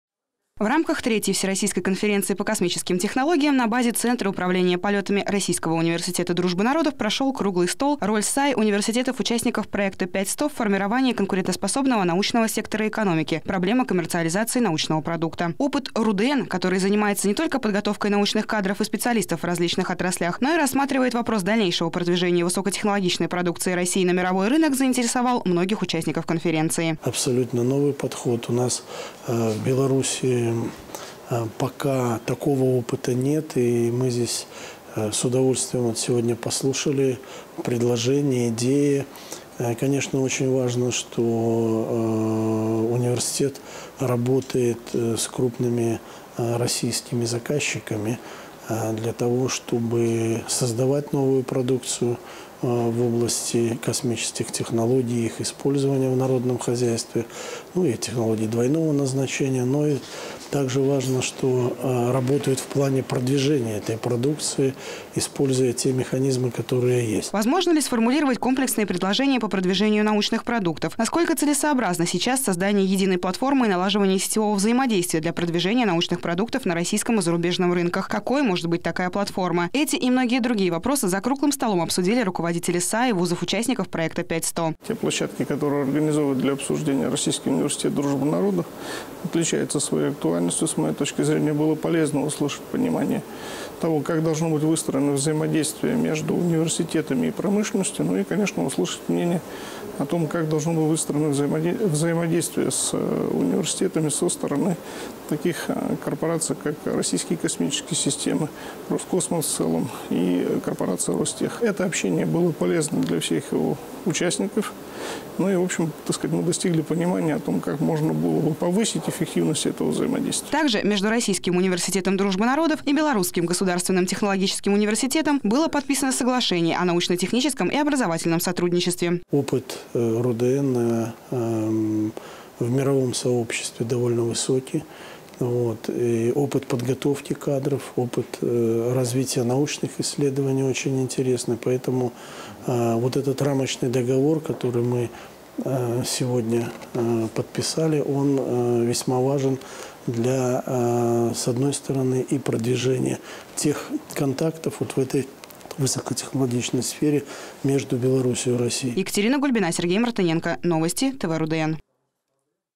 Круглый стол в рамках «Недели космических технологий» в РУДН
В рамках третьей всероссийской конференции по космическим технологиям на базе Центра управления полетами Российского университета дружбы народов прошел круглый стол «Роль САЕ университетов-участников проекта «5-100» в формировании конкурентоспособного научного сектора экономики. Проблема коммерциализации научного продукта». Опыт Российского университета дружбы народов, который занимается не только подготовкой научных кадров и специалистов в различных отраслях, но и рассматривает вопрос дальнейшего продвижения высокотехнологичной продукции России на мировой рынок, заинтересовал многих участников конференции.